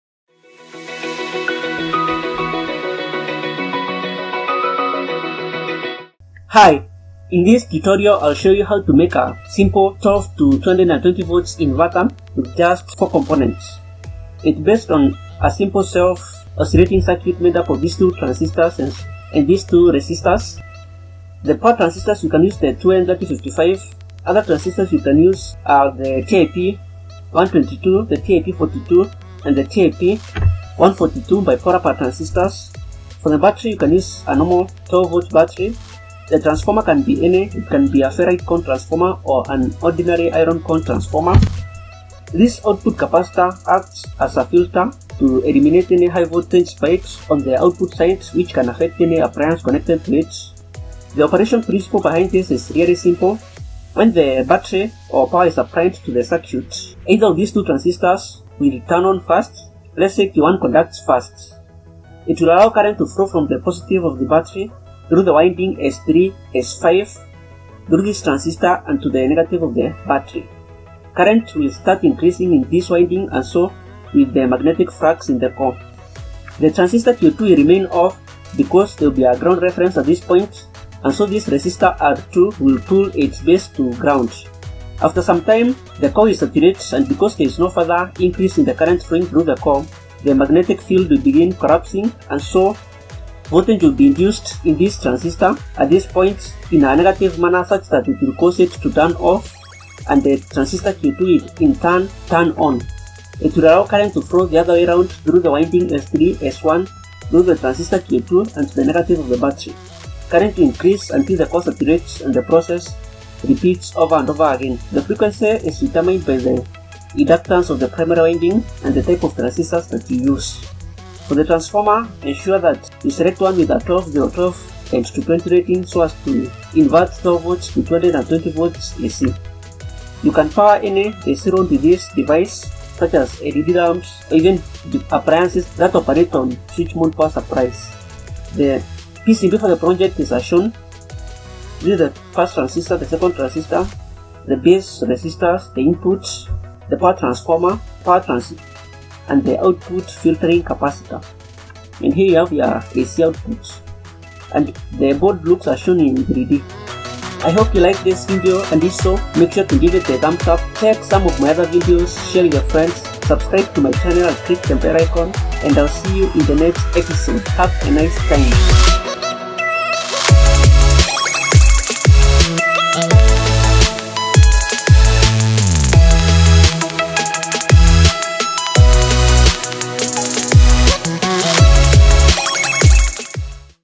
Simplest Inverter Circuit you can Make (12v to 220v DC-AC) high frequency Pure sine wave
Hi. In today's video I'll show you how to the simplest Inverter with just five components. It's based on a self Oscillating circuit with two Bipolar Transistors.